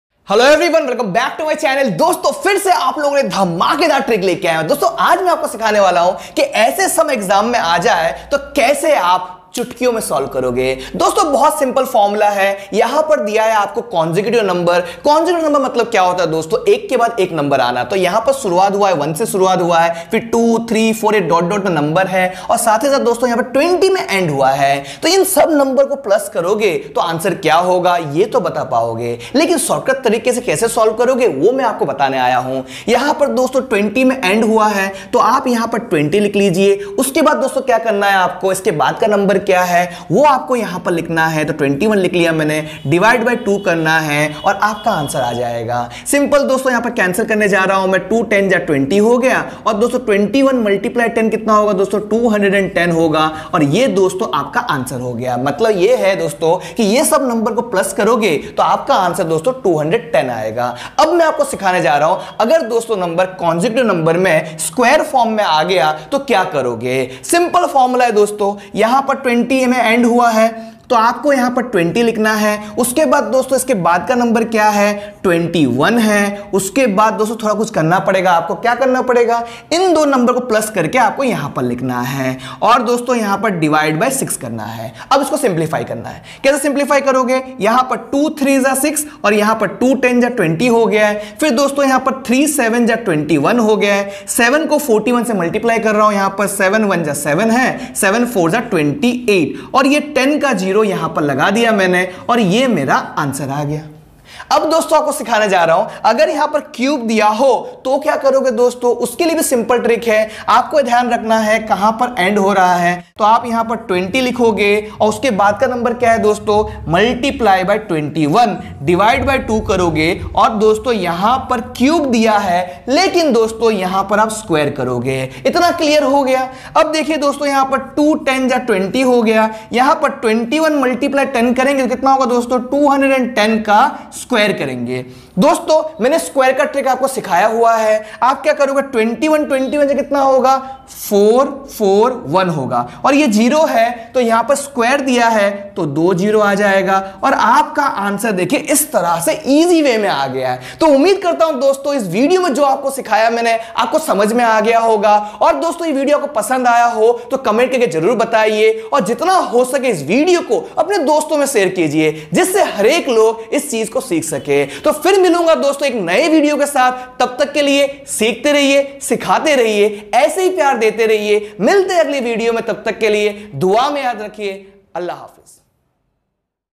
Maths Short Tricks | Sum of natural numbers | Addition Tricks | imran sir maths
Hello Friends
In this video following points will be covered
1.Sum of natural numbers Trick
2.How to add consecutive numbers easily
3. Shortcut Trick for addition
4. Best trick for consecutive square and cube numbers
5. Maths trick by imran sir

Here you all will get many more tricks which are going to help in your Competitive Exams